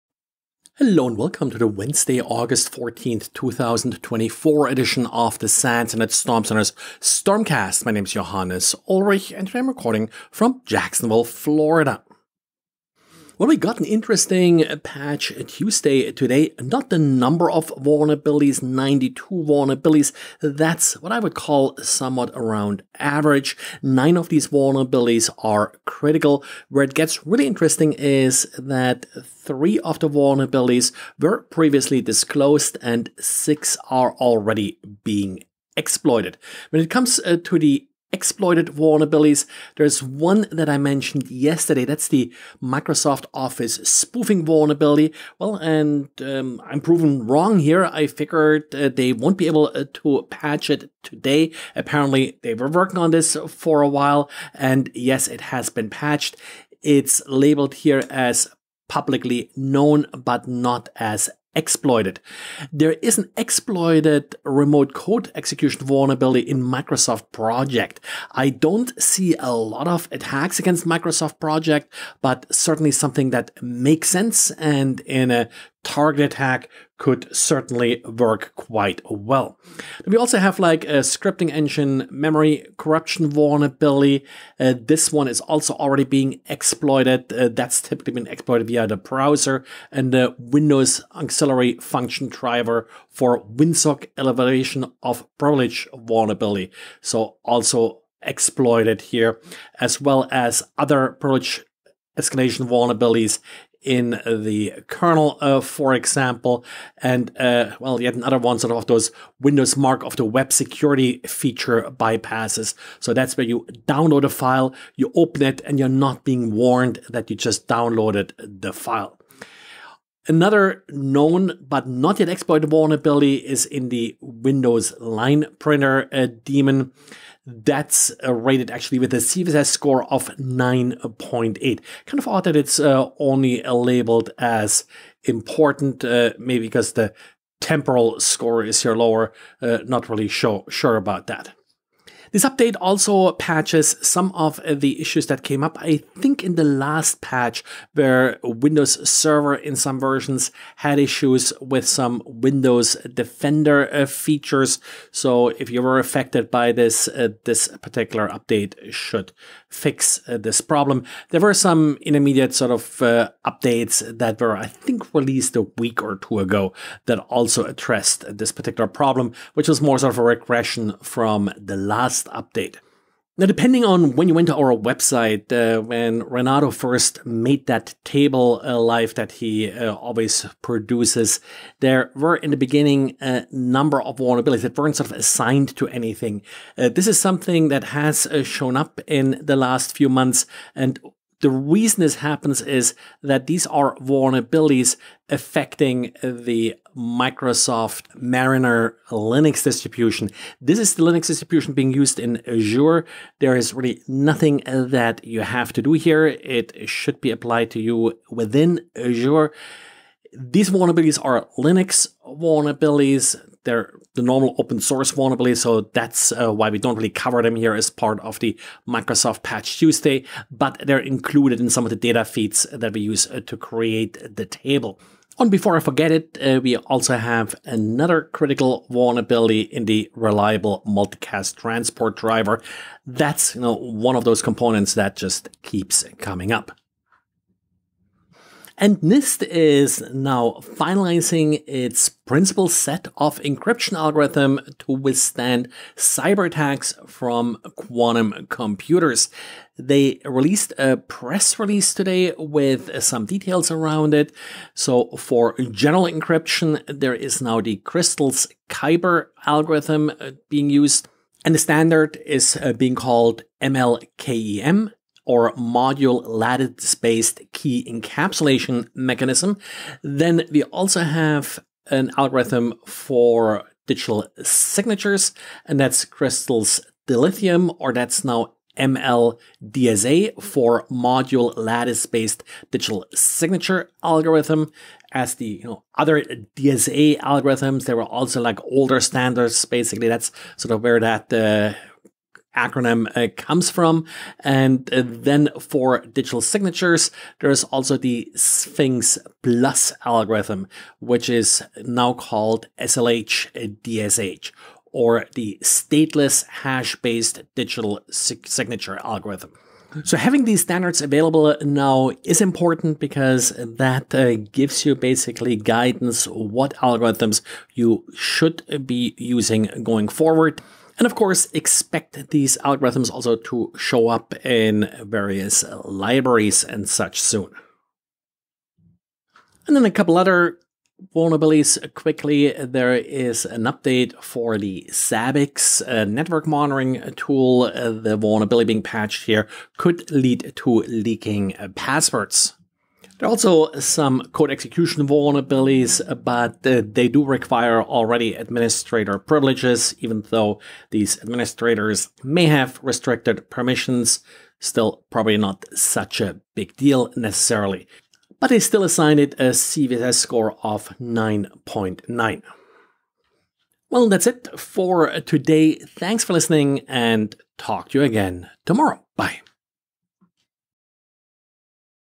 Network Security News Summary for Wednesday August 14th, 2024
Microsoft Patches; Post Quantum Encryption; Zabbix Vulns;

 (and others)